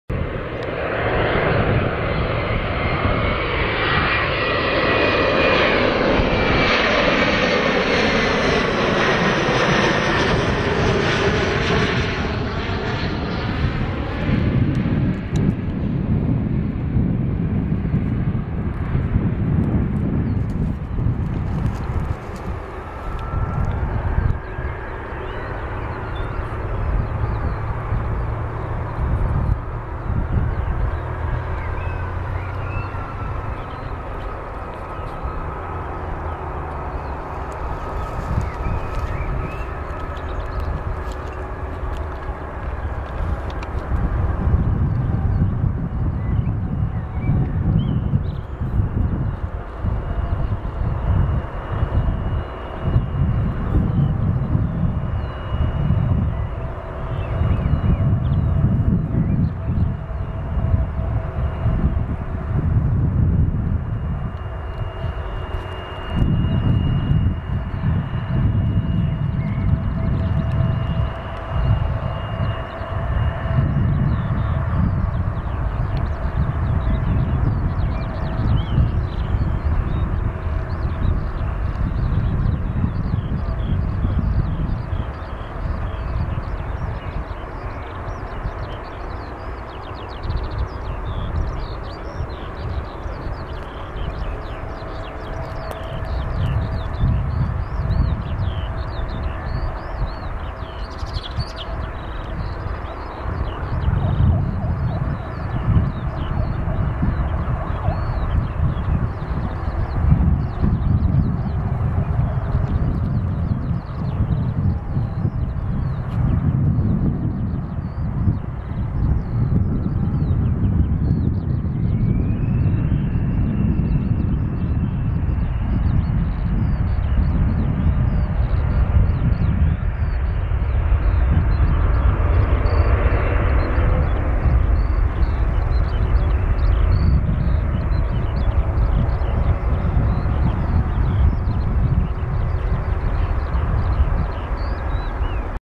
EPKK Spotting 05.04.09
Spotting at Balice Airport. Aircraft: SP-LDC Embraer ERJ-170 LOT Polish Airlines , EC-JZT CRJ-900ER Air Nostrum (Iberia Regional), SP-LPA Boeing 767-300ER LOT Polish Airlines. Date: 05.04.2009